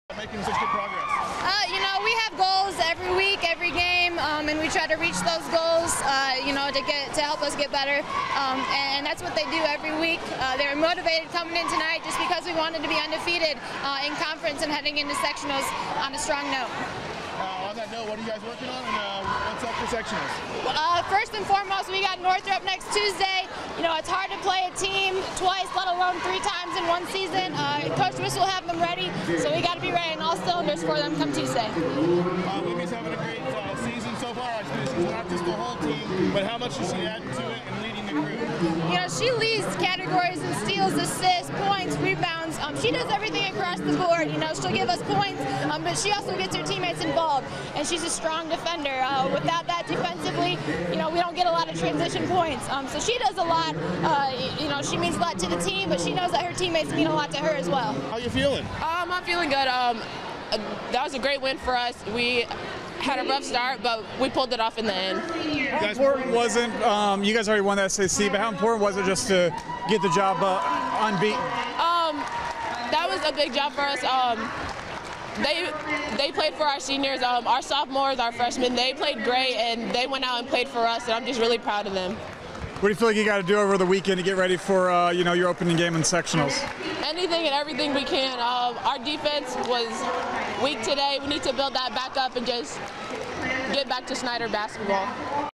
Extended Snider girls post game
Extended interviews with Snider girls after their win over Luers. The Panthers went a perfect 7-0 in SAC this year.